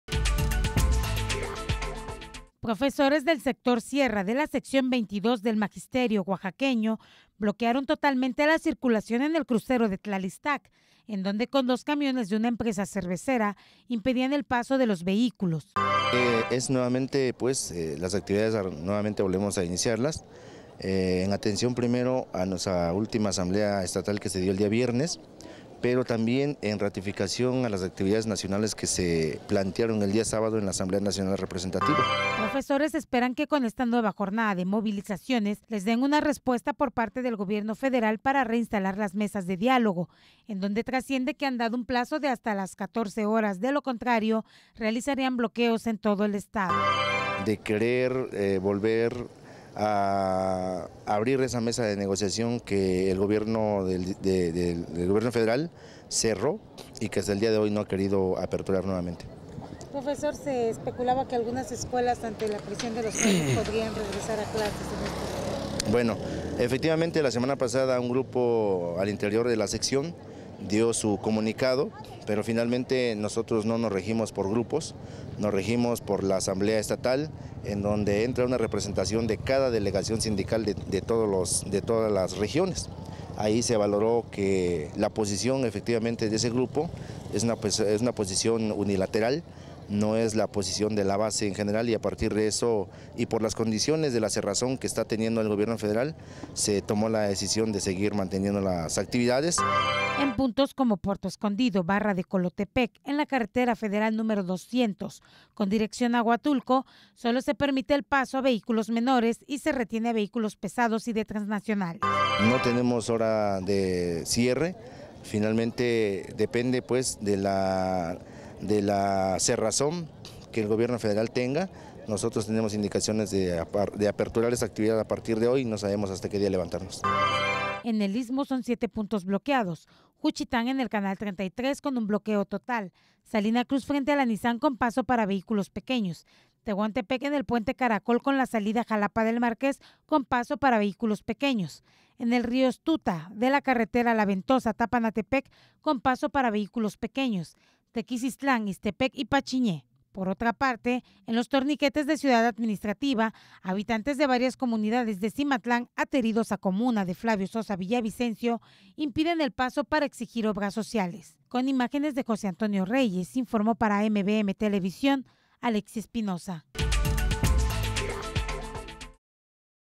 Profesores bloquean crucero de Tlalixtac.
Profesores del Sector Sierra de la Sección XXII del magisterio oaxaqueño, bloquearon totalmente la circulación en el crucero de Tlalixtac, en donde con dos camiones de una empresa cervecera, impedían el paso de los vehículos.